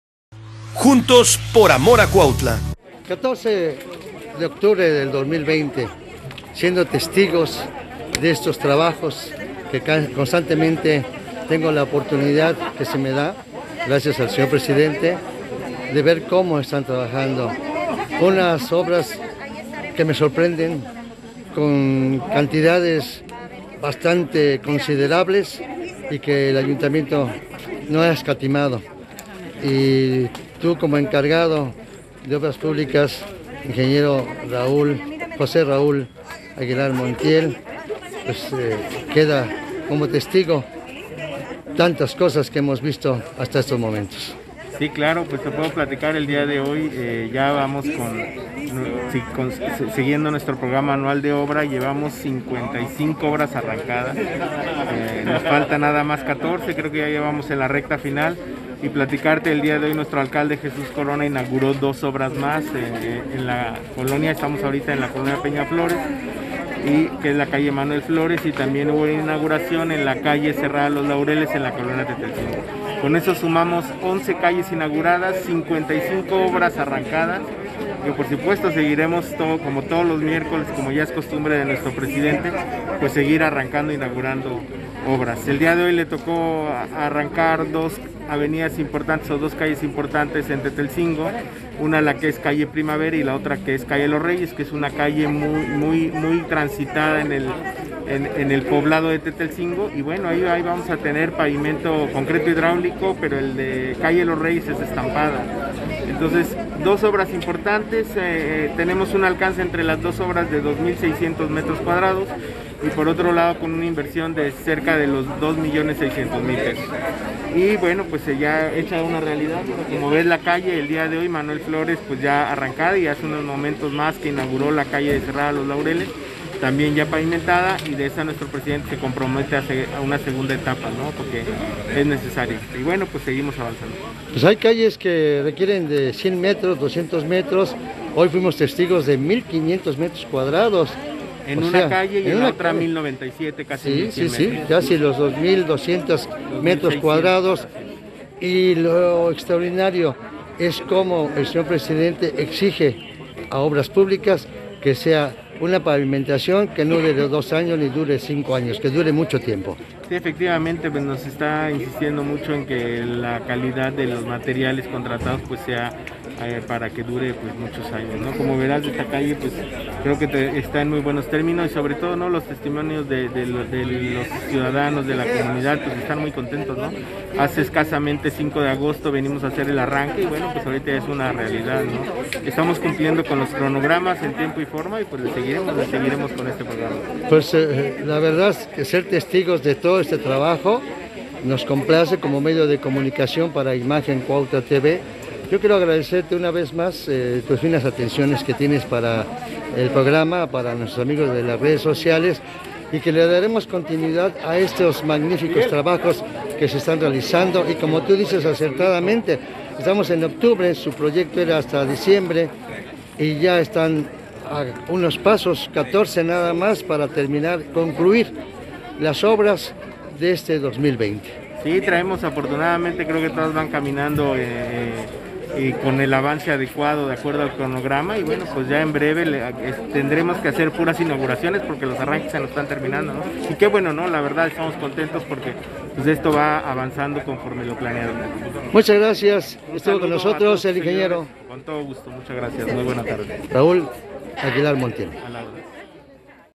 ING. JOSE RAUL AGUILAR MONTIEL, NOS PLATICA QUE SOLO FALTAN 14 OBRAS PARA CUMPLIR ESTE 2020
ING. JOSE RAUL AGUILAR MONTIEL, NOS PLATICA QUE SOLO FALTAN 14 OBRAS PARA CUMPLIR ESTE 2020 EN LOS OBJETIVOS DE REALIZAR 69 OBRAS